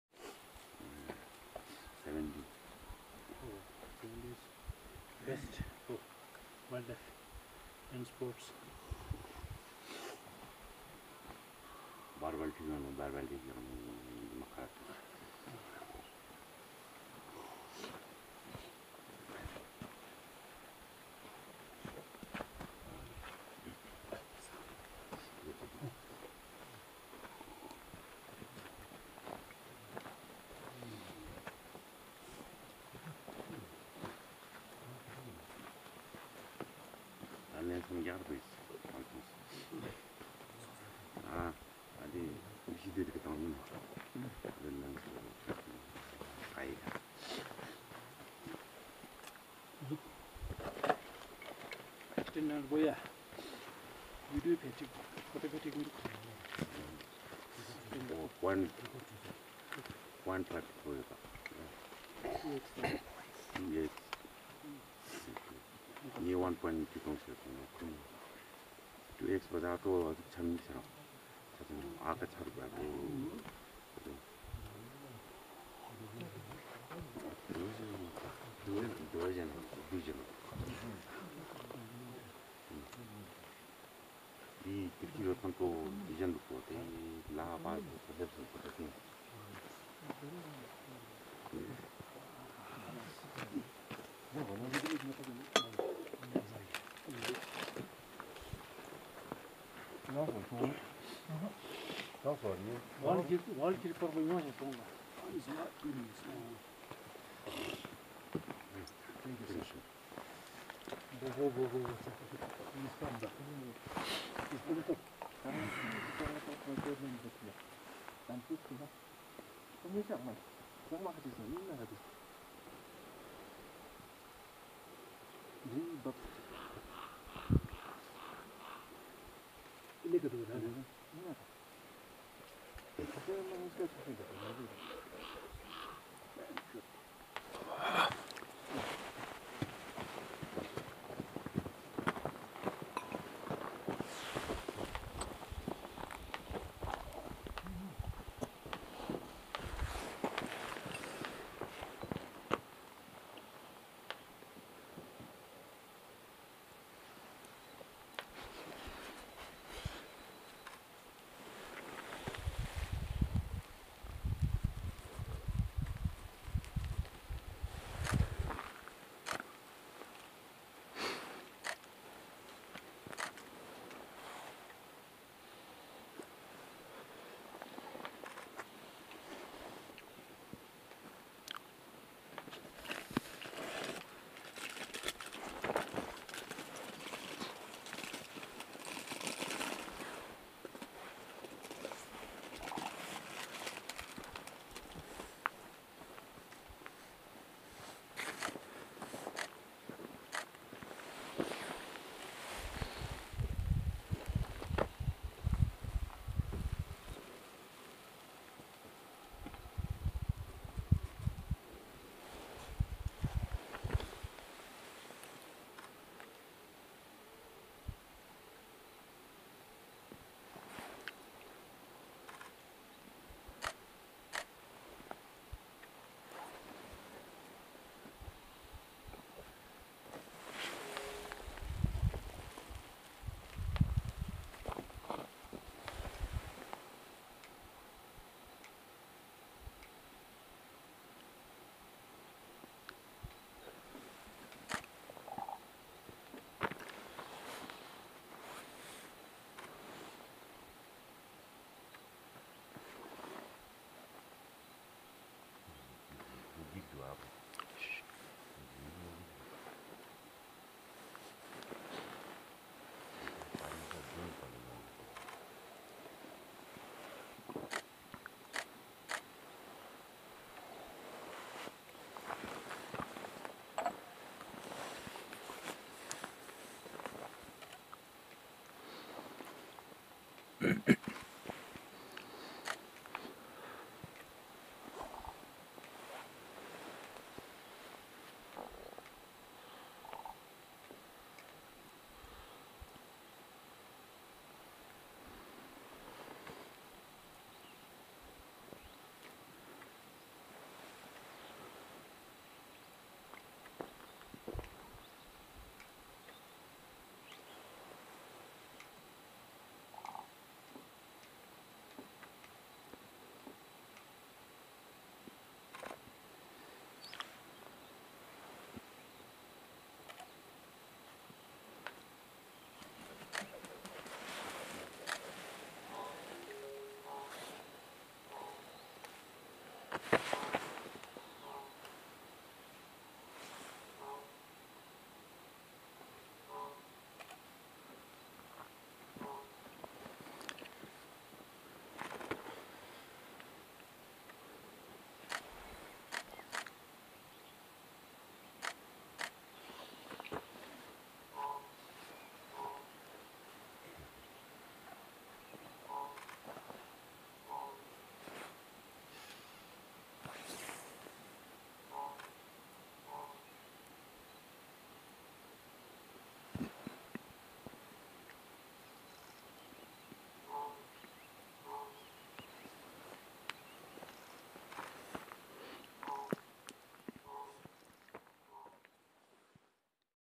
An interaction between Eurasian Eagle Owl and Northern Raven Ladakh India
The Eurasian eagle-owl (Bubo bubo) is a species of eagle-owl that resides in Eurasia ,It is one of the largest species of owl, and females can grow to a total length of 75 cm (30 in), with a wingspan of 188 cm (6 ft 2 in), males being slightly smaller.This bird has distinctive ear tufts, with upper parts that are mottled with darker blackish colouring and tawny. The wings and tail are barred. The underparts are a variably hued buff, streaked with darker colour. The facial disc is not very visible and the orange eyes are distinctive.